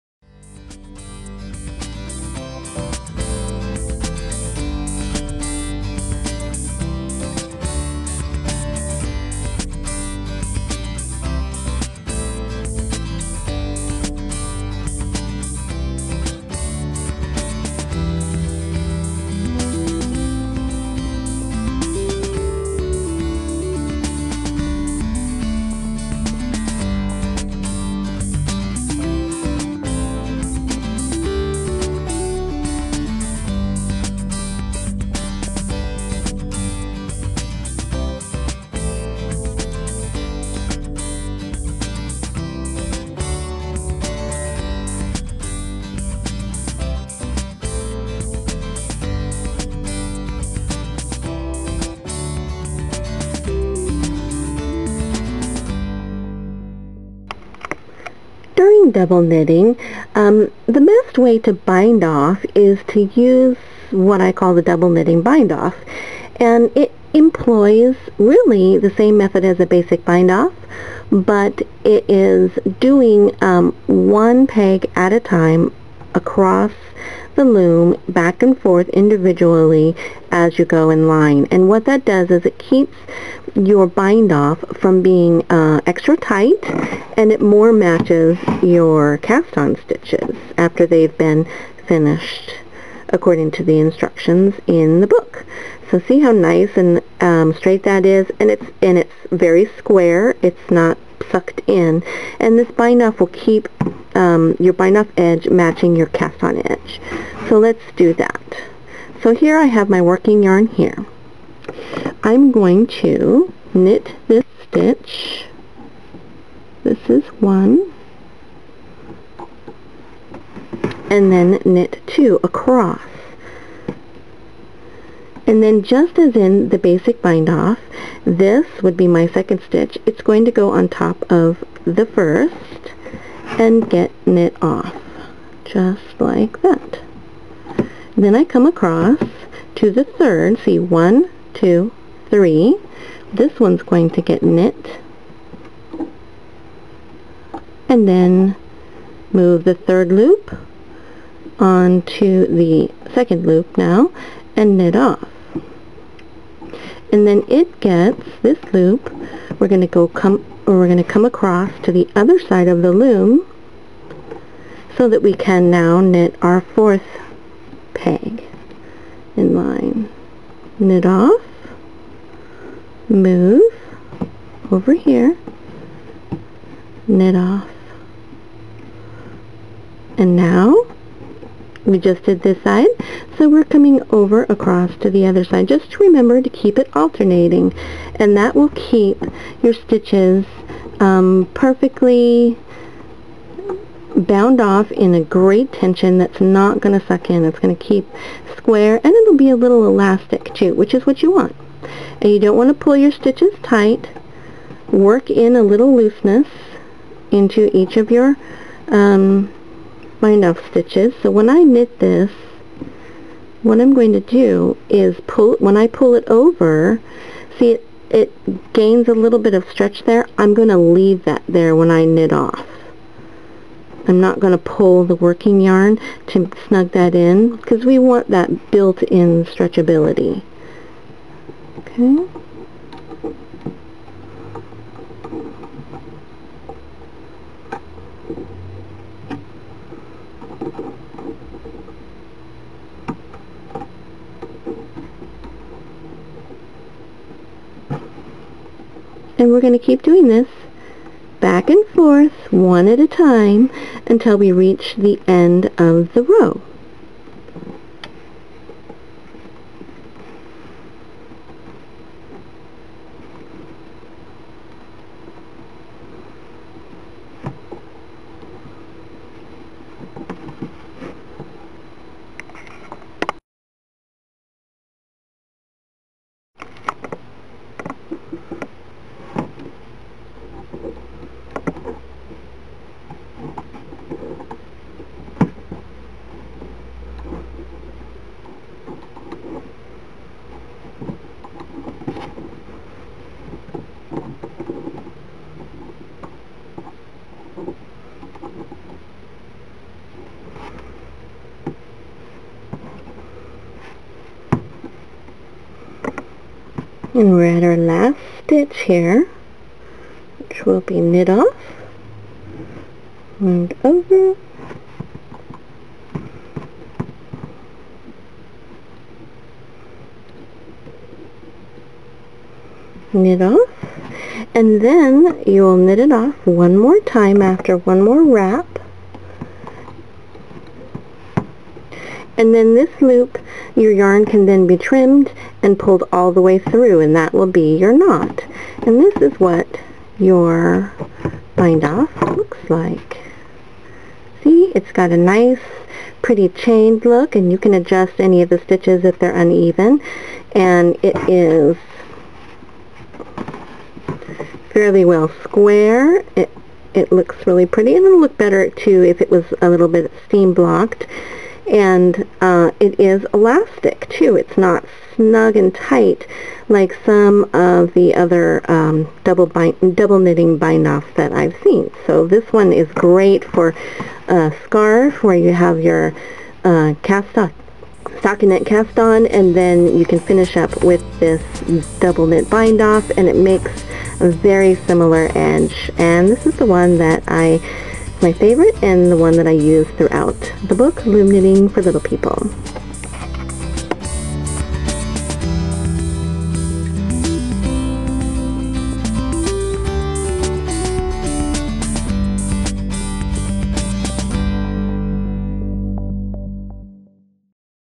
Loom Knitting- Double Knitting Cast On and Bind Off
Learn to easily Cast On to a knitting board or long loom!  Then, when you have that mastered, learn to Bind Off your double knitted project by using a technique that results in a stretchy bind off edge that matches your Cast On!